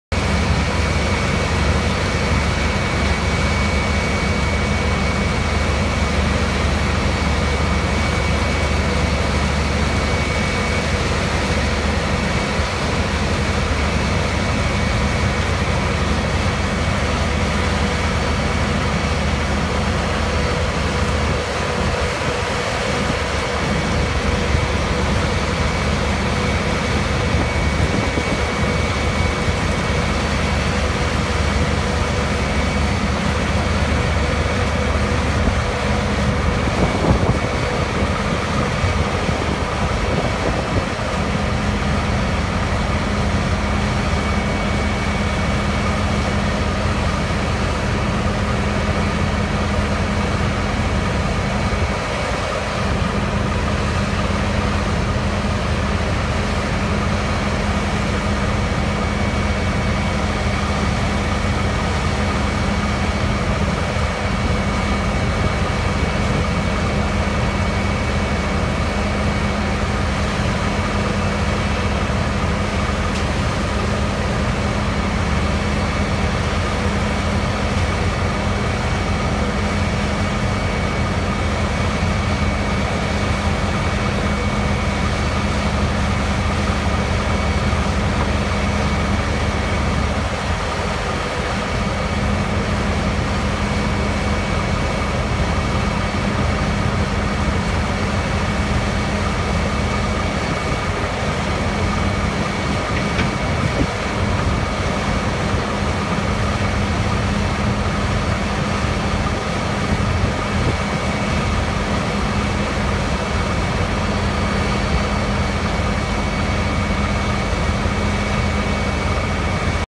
Powerscreen Trakpactor 320SR crushing asphalt
Powerscreen Trakpactor 320SR closed circuit impactor crushing millings to 1/2" minus